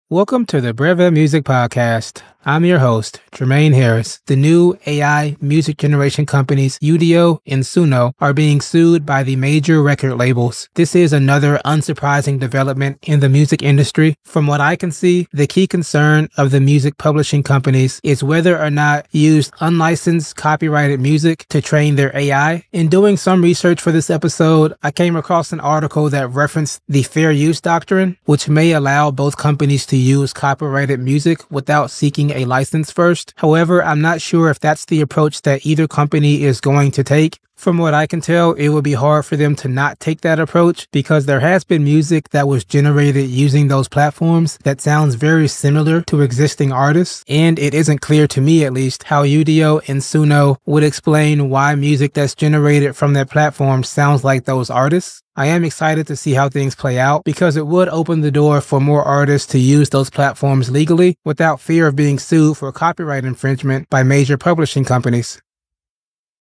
49: Udio and Sumo Lawsuits
SummaryIn the Breve Music Podcast hosted by Jermaine Harris, the discussion revolves around the legal issues facing AI music generation companies UDO and SUNO, who are being sued by major record labels for potentially using unlicensed copyrighted music to train their AI systems. The key concern for music publishing companies is whether these companies are infringing on copyrights. Despite the uncertainty, the outcome of this legal battle could pave the way for more artists to utilize such platforms legally without fear of copyright lawsuits from major publishing companies.